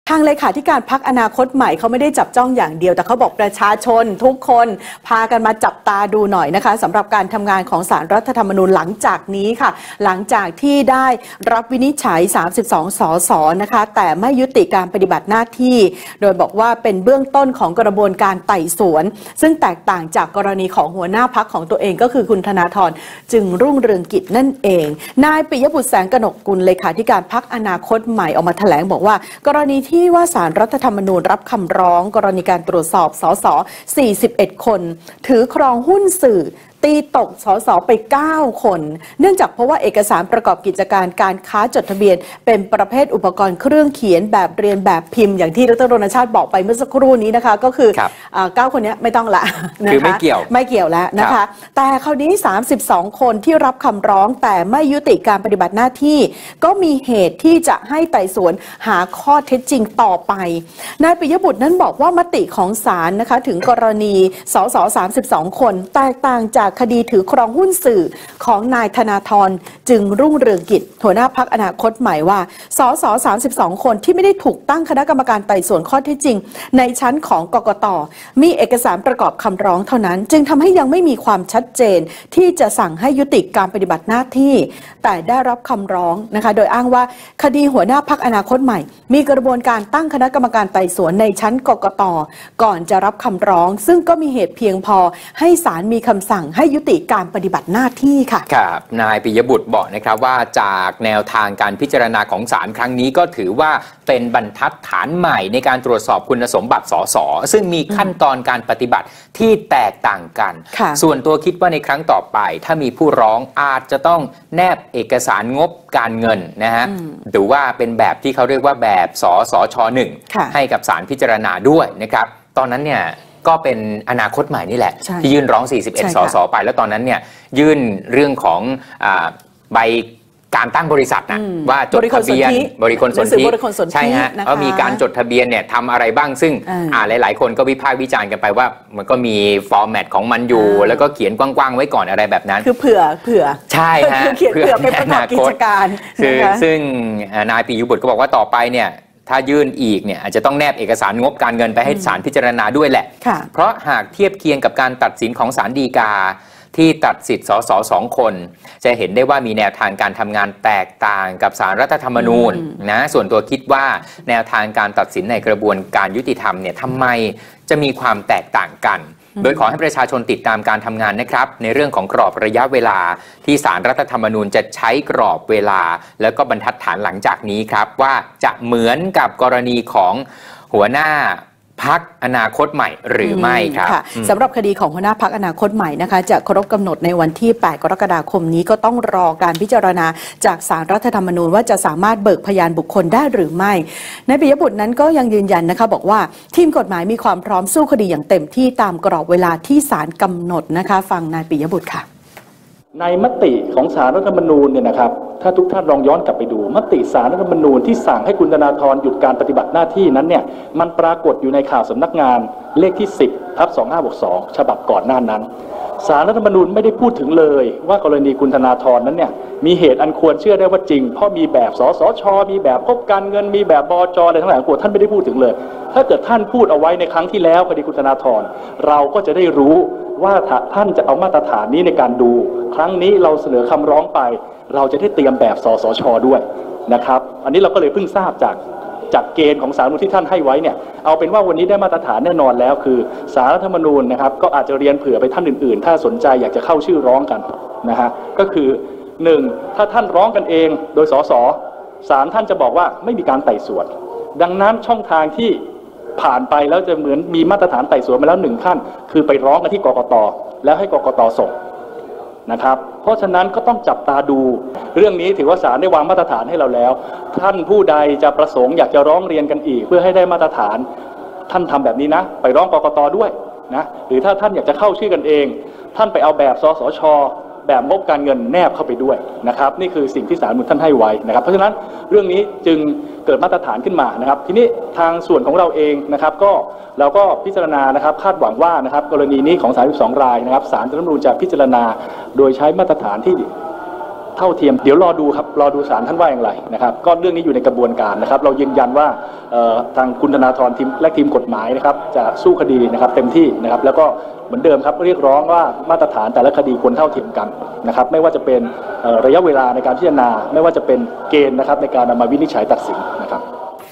"ปิยบุตร" แถลง ศาลฯรับวินิจฉัย 32 ส.ส. แต่ไม่ยุติหน้าที่
27/06/2019 "ปิยบุตร" แถลง ศาลฯรับวินิจฉัย 32 ส.ส. แต่ไม่ยุติหน้าที่